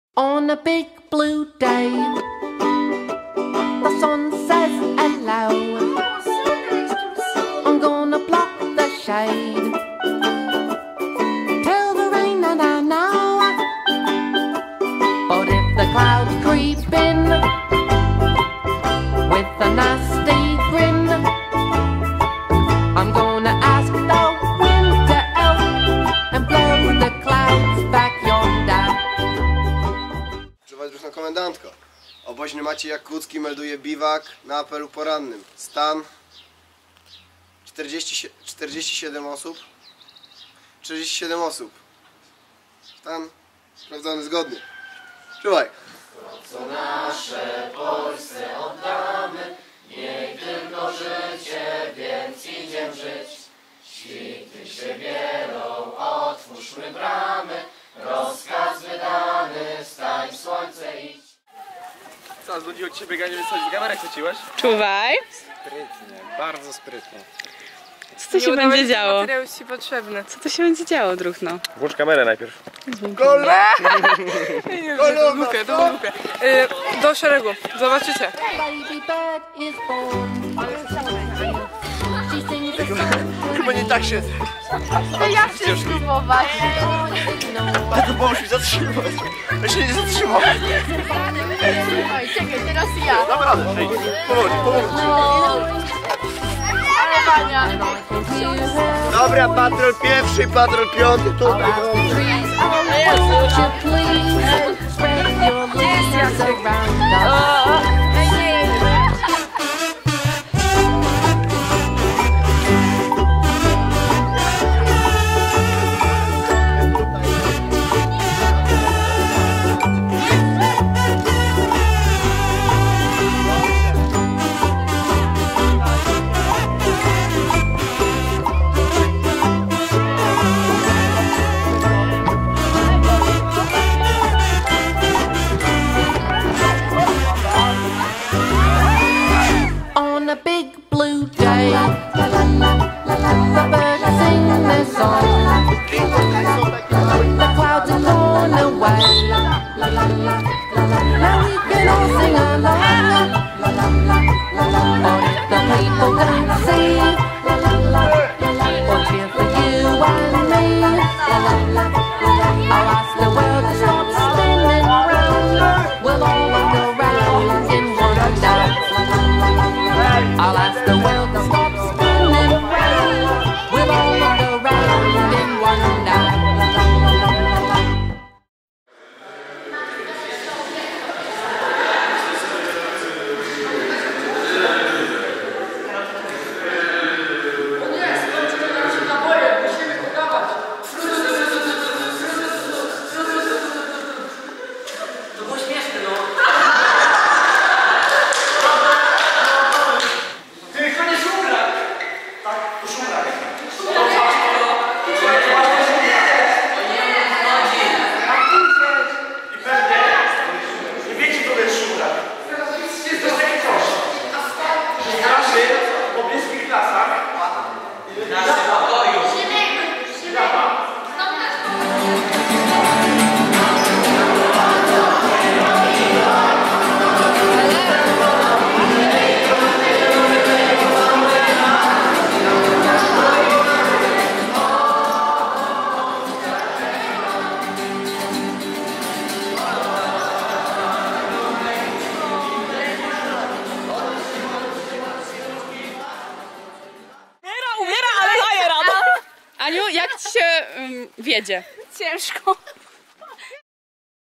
Biwak - Laszczyny 2013
Specjalnie dla was relacja z wiosennego biwaku Hufca ZHP Leżajsk, który odbył się w Laszczynach 14-16 czerwca br. :)